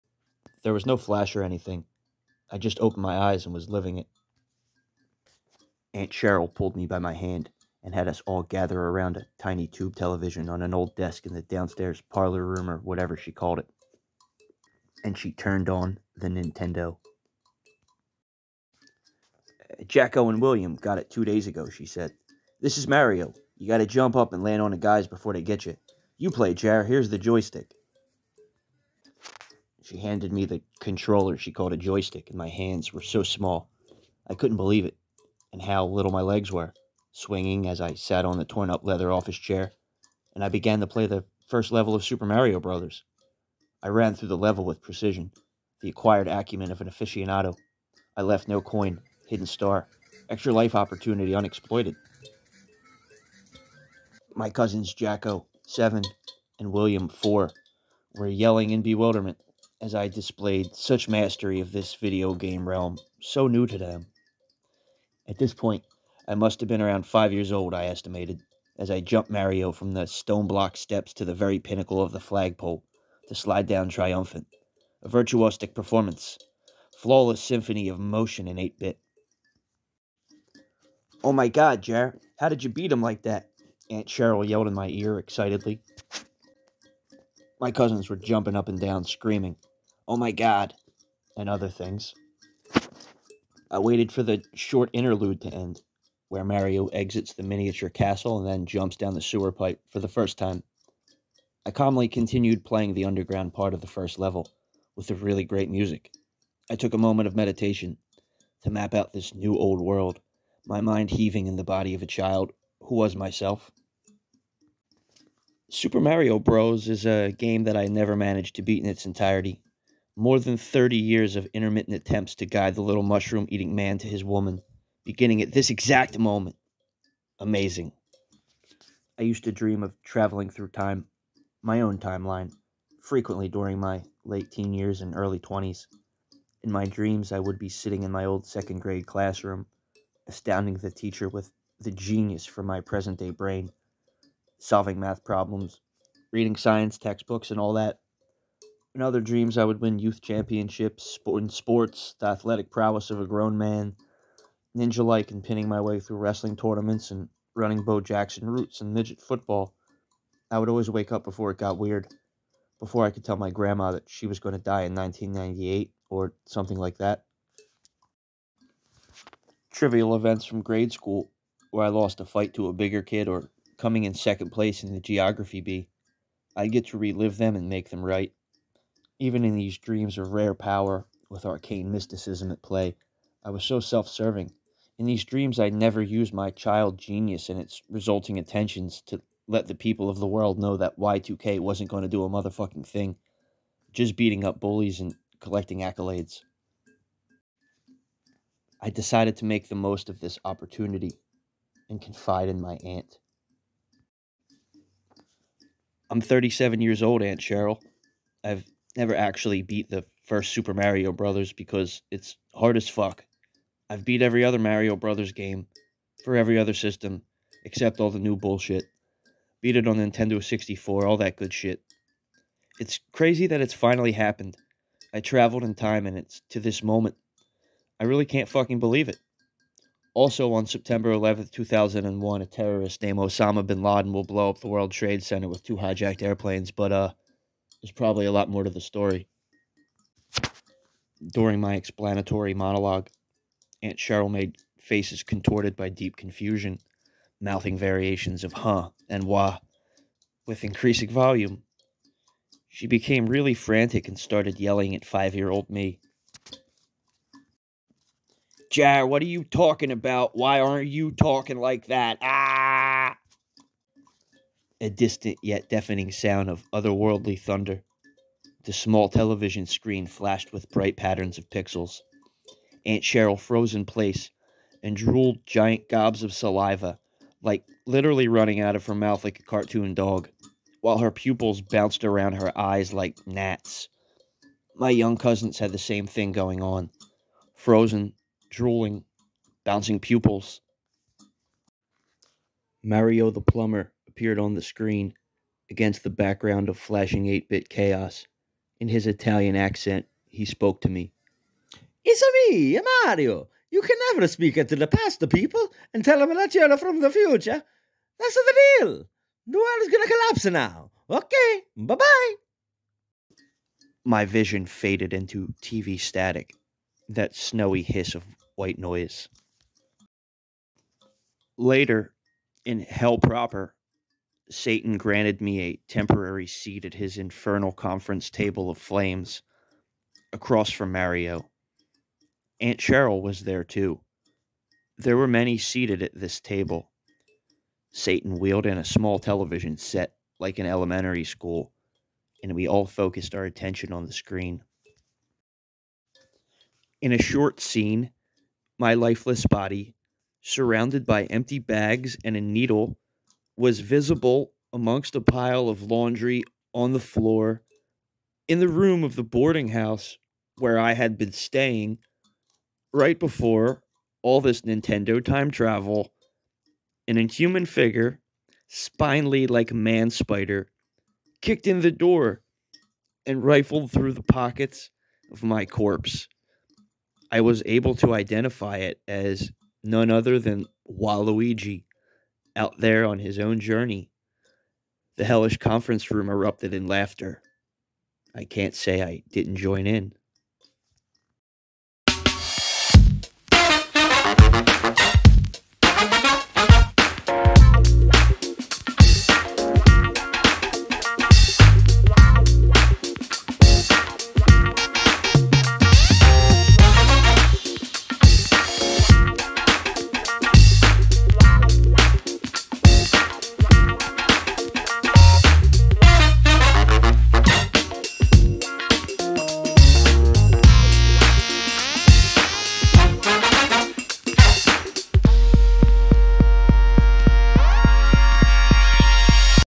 Ring of Hell: 1990 by Marty Flytrap
Narrated by Marty Flytrap, assembled video collage of memeGIFs from the World Wide Web

Featuring Kevin Mann on audio/visual